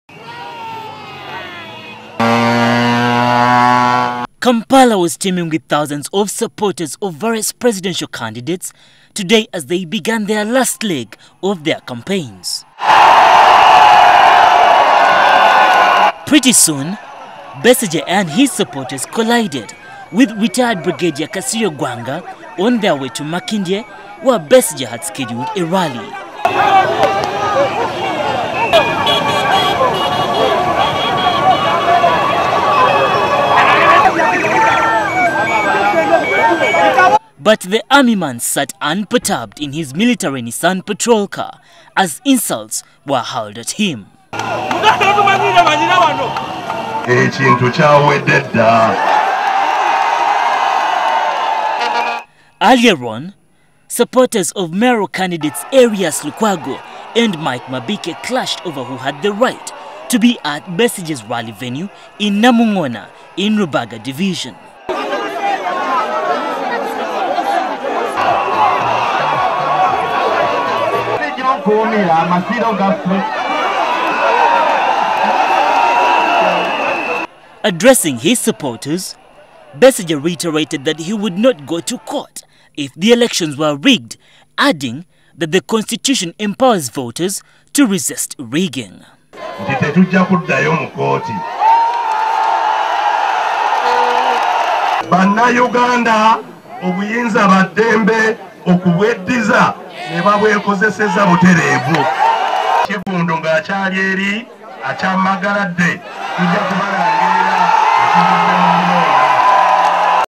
Besigye cautions Electoral Commission
IPC candidate Dr Kiiza Besigye had a brief confrontation with retired army officer Brigadier Kasirye Ggwanga, as the IPC presidential candidate was on the campaign trail at Makindye here in Kampala.